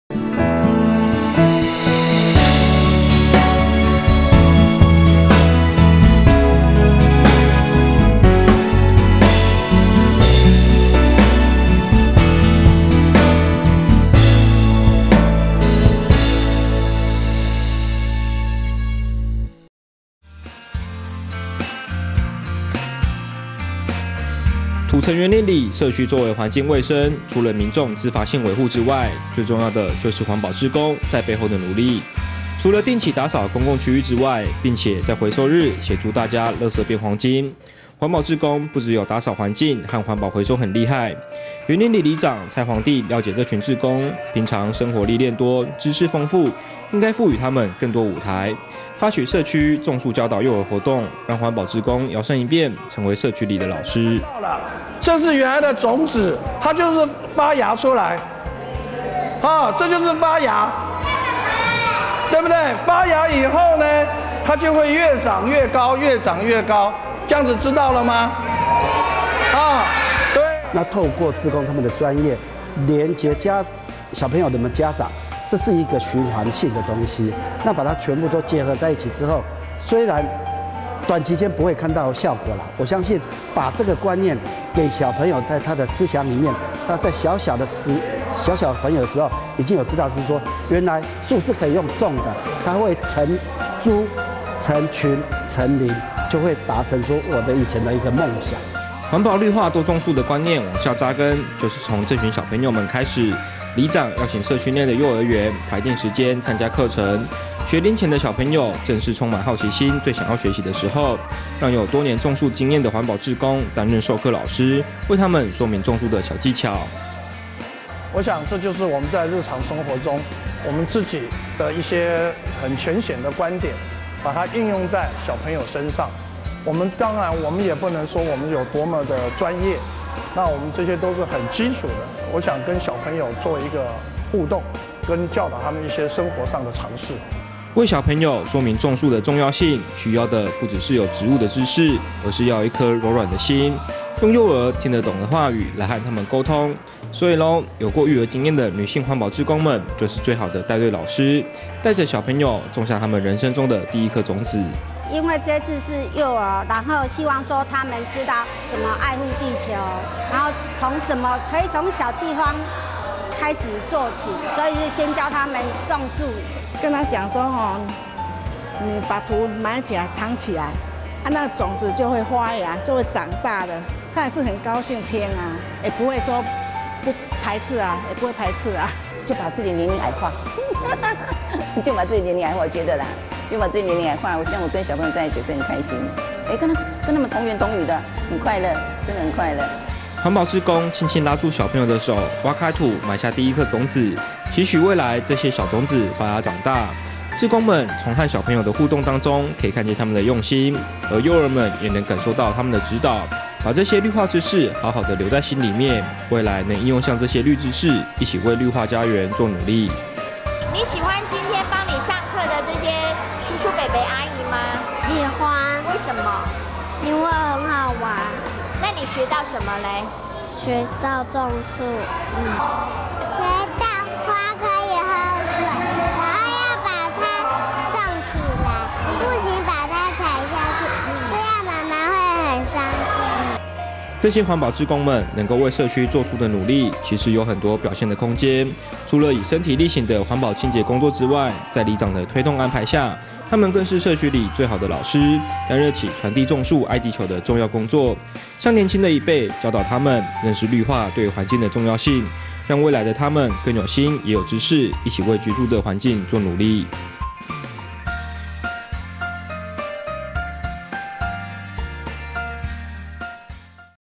1061026【中嘉新聞】員林里志工化身老師 教幼兒種樹綠美化
員林里志工化身老師 教幼兒種樹綠美化